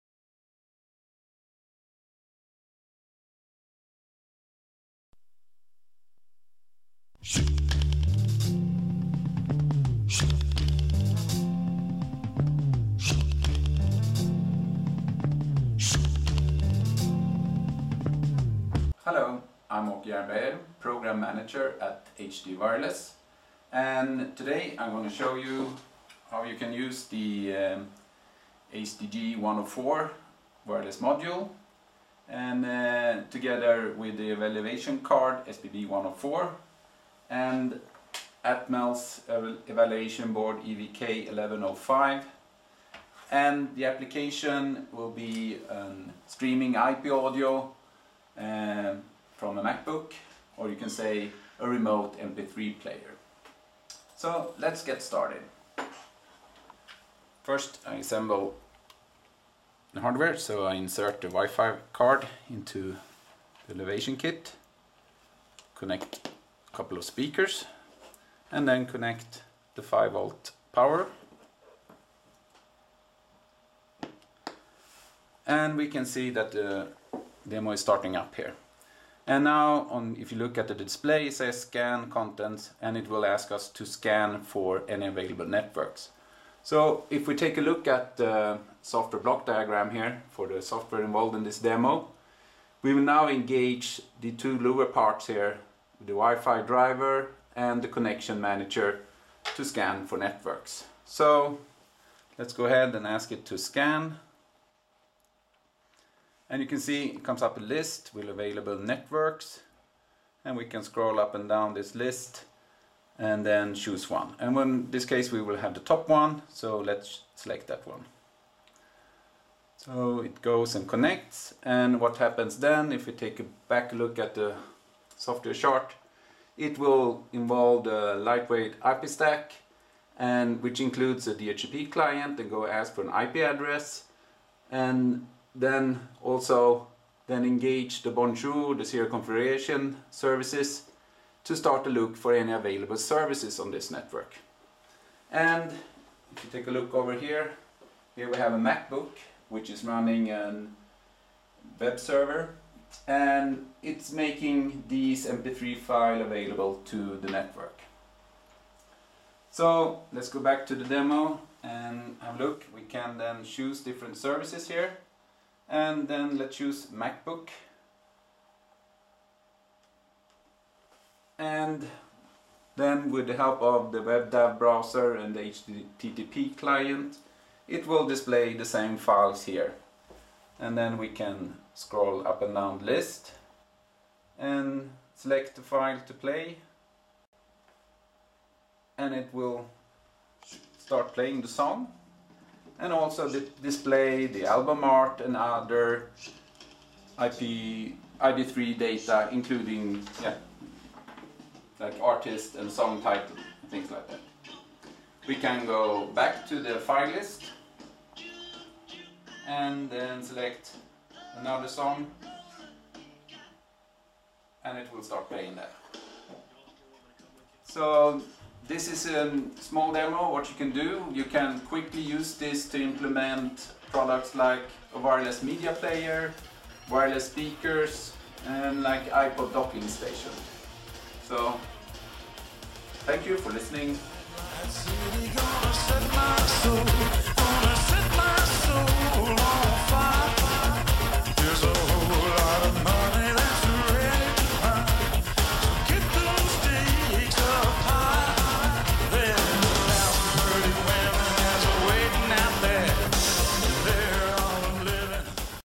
H&D Wireless AB - Streaming Audio Demo over Wi-Fi !
EVK1105 (32bit AVR) and Wi-Fi SPB104 streaming audio - remote mp3 player proof of concept solution. Engineering video.